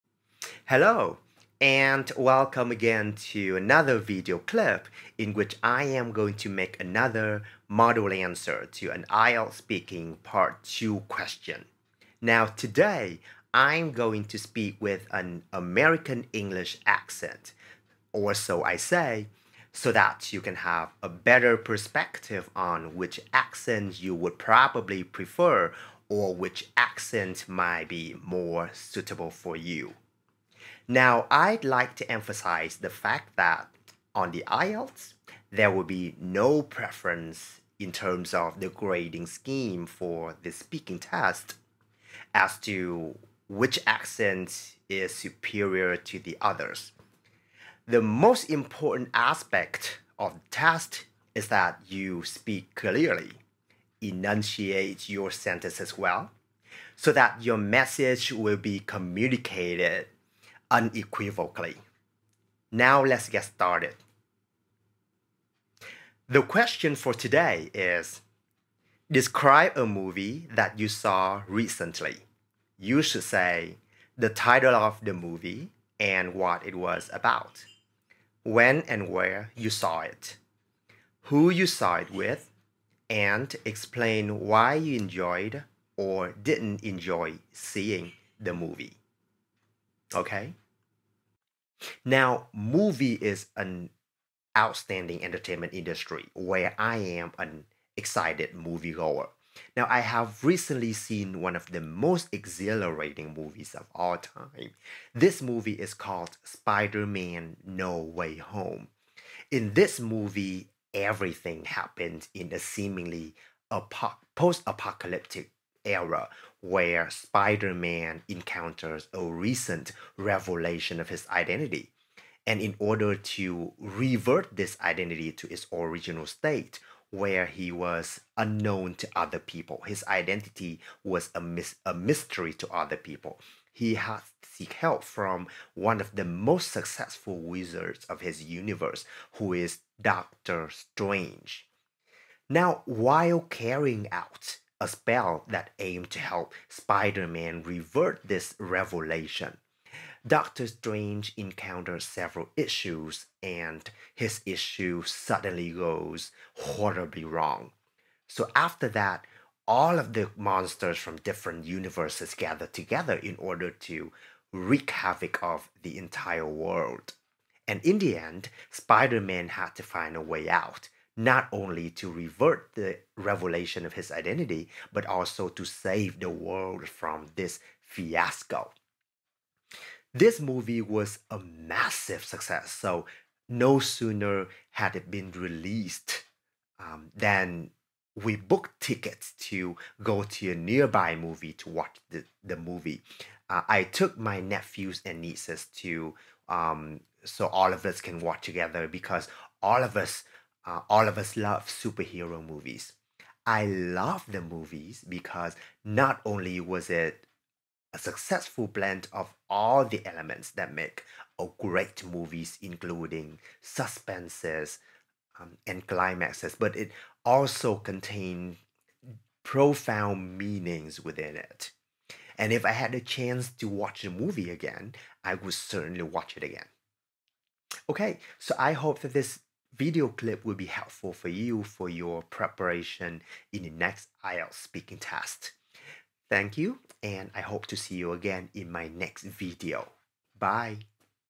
[Hung Pham EDU] IELTS Speaking - Part 2 Model Answer - Recent Movie (with In-Video Annotation)
Another example of an IELTS Speaking Part 2 question, explanation of  IELTS speaking elements of grading criteria explained inside the video.

Describe a movie that you saw recently. You should say:

- The title of the movie and what it was about
- When and where you saw it
- Who you saw it with

And explain why you enjoyed/didn't enjoy seeing the movie